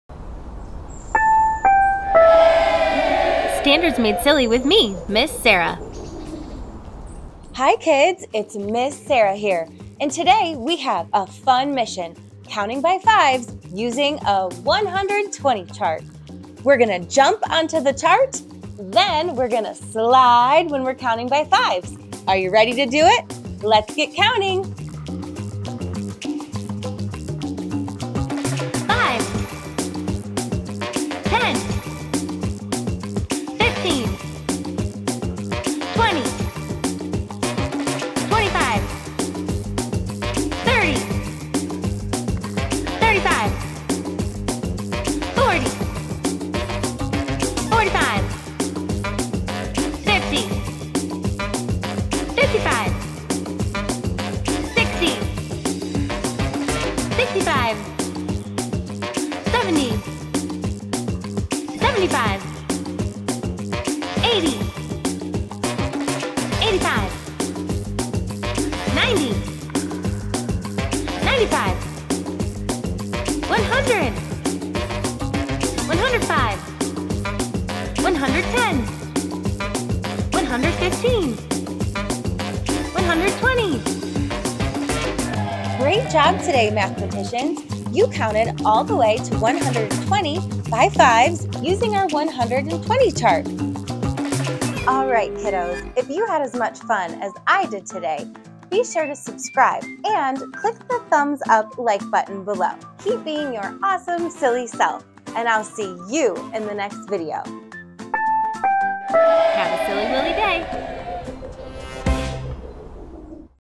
Counting by 5s to 120 with Ms. Sarah!
Blast off on an epic counting adventure with Ms. Sarah! "Galactic Jump: Counting by 5s" invites young learners to explore a space cave on a mission to count to 120 using a 120 chart. Each leap by 5 reveals a universe filled with numbers, lighting up the path to mathematical mastery.

👩‍🚀💫 What cosmic counting awaits?

Join Ms. Sarah as she hops from one number to the next on a glowing 120 chart. Each number in the sequence of 5s shines brightly, marking our journey through the mission.

Experience the thrill of counting by 5s, as every jump illuminates a new step in the sequence, making learning to count not only educational but visually stunning.

This odyssey is not just a journey; it's a fun, engaging way to achieve the learning target: "I can count to 120 by 5s using a 120 chart," aligning perfectly with standard 1.NBT.A.1.

📚 Why embark on this galactic adventure?

- Designed to meet the 1.NBT.A.1 standard, ensuring a focused educational experience that's both fun and effective.
- Crafted by Ms. Sarah, who combines her passion for teaching with innovative methods to make complex concepts accessible to young learners.
- The use of a 120 chart and visual cues supports understanding of numerical order and pattern recognition, essential skills for budding mathematicians.
- Ideal for educators and parents seeking engaging, educational content to support their children's mathematical development.

🚀 Ready to make every count count? Join Ms. Sarah on this luminous journey through numbers and space. This video is more than a lesson; it's an invitation to explore, learn, and love math.

Together, let's light up the universe of mathematics for our young space explorers!